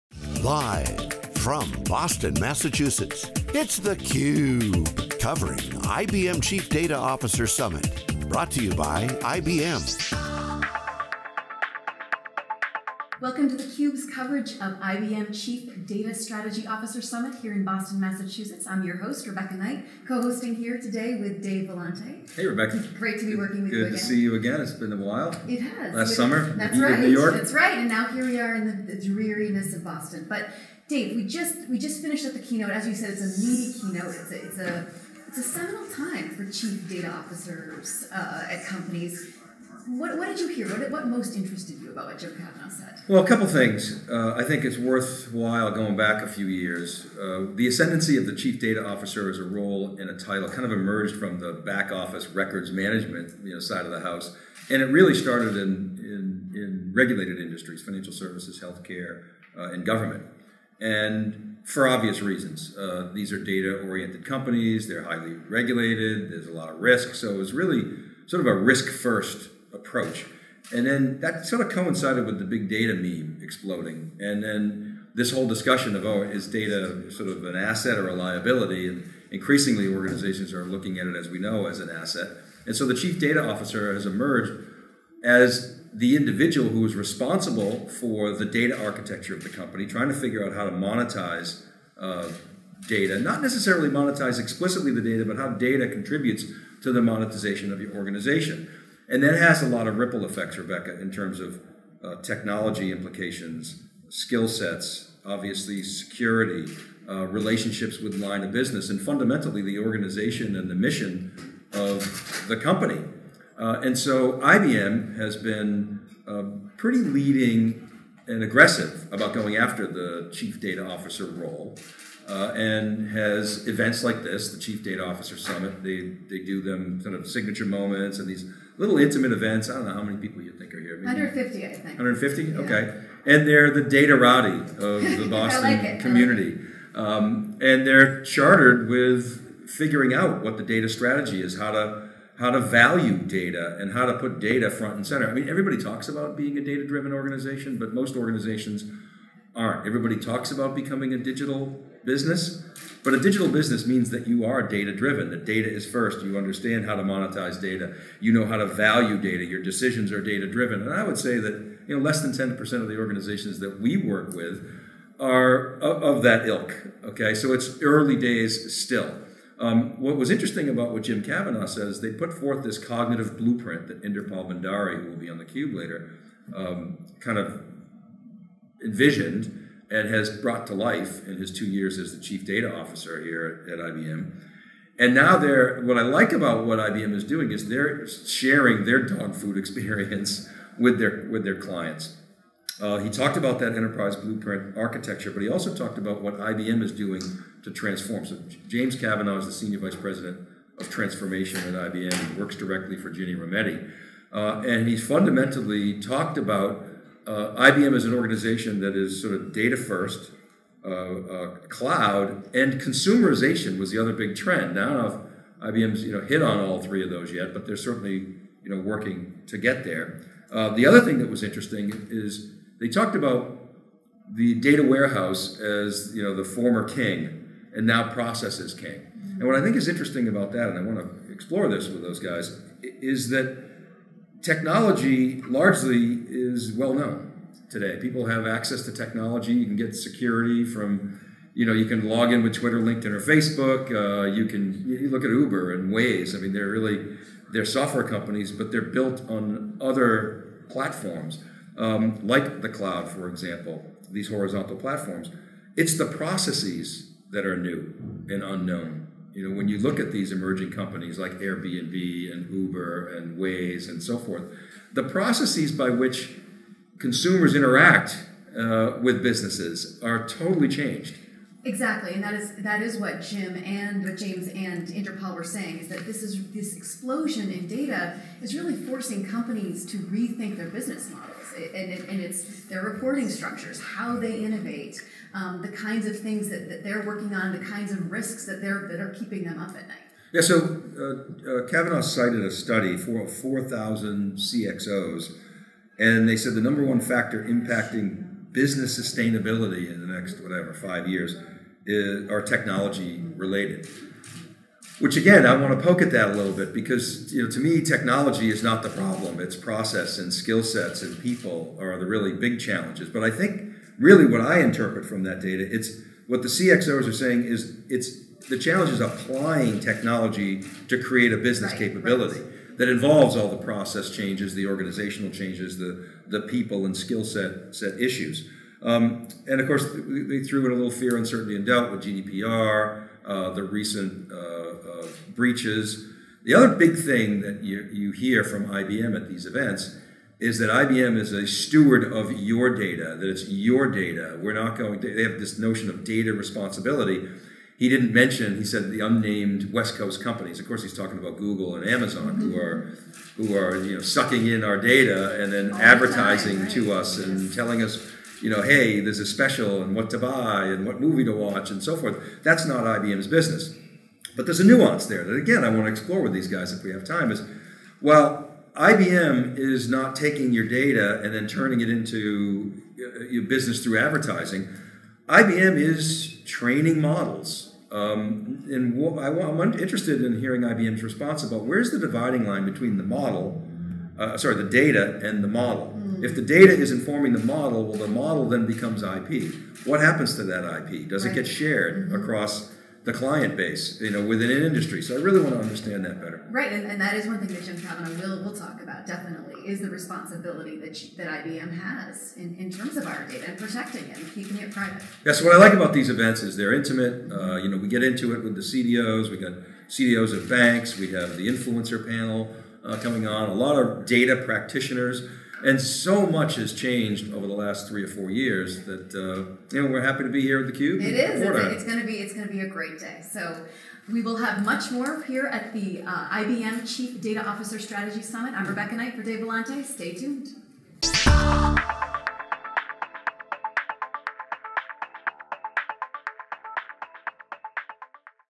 Kickoff | IBM CDO Strategy Summit 2017
Dave Vellante and Rebecca Knight kickoff IBM CDO Strategy Summit 2017 at the Boston Park Plaza Hotel in Boston, MA